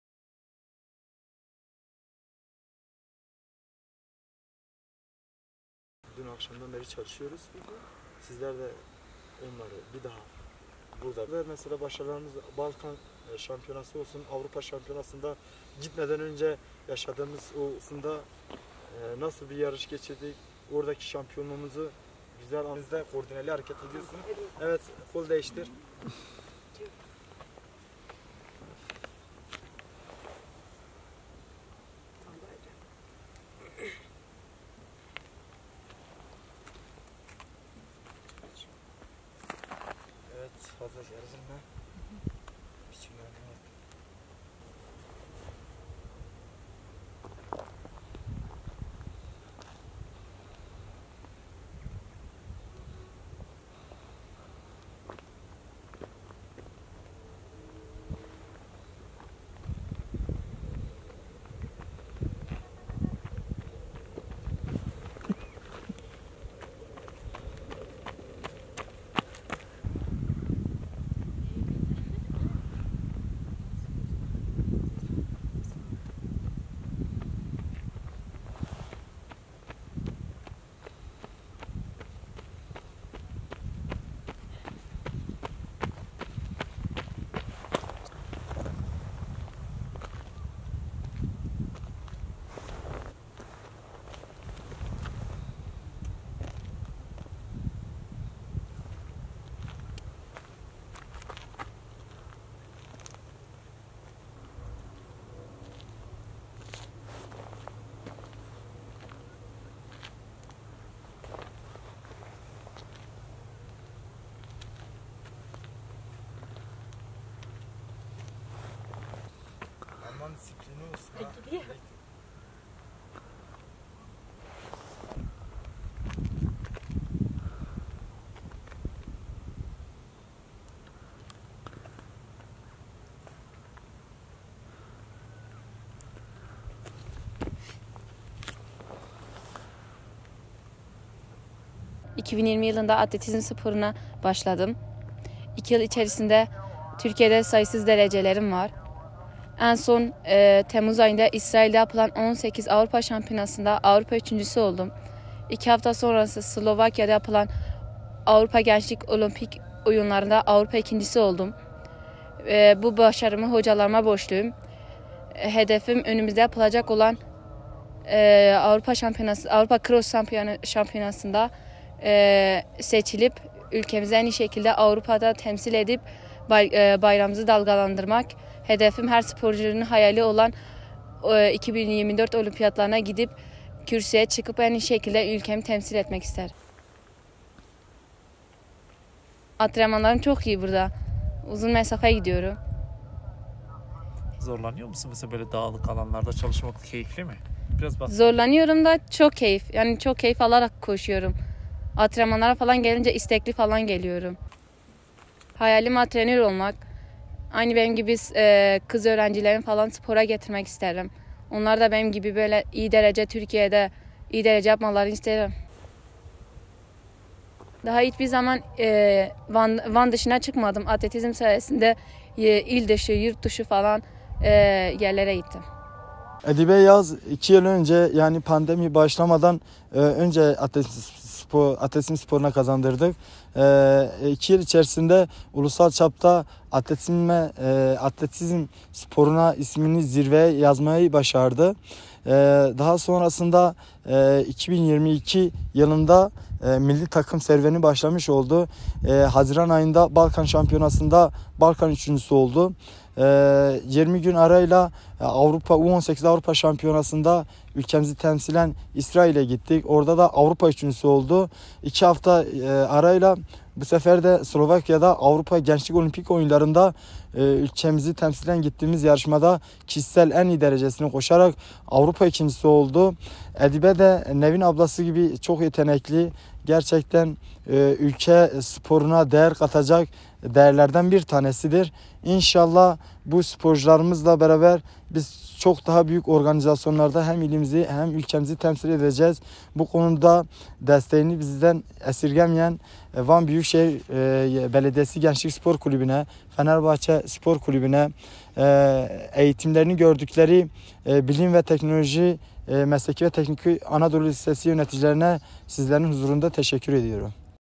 Van'ın yüksek rakımlı tepelerinde olimpiyat hedefiyle çalışıyor
2022 Avrupa Gençlik Olimpik Yaz Festivali'nde kadınlar 3000 metrede ikinci olan 16 yaşındaki Edibe Yağız, 2024 Olimpiyatları'na katılmak ve altın madalya kazanmak hedefiyle yüksek rakımlı tepelerde kondisyon antrenmanları yapıyor.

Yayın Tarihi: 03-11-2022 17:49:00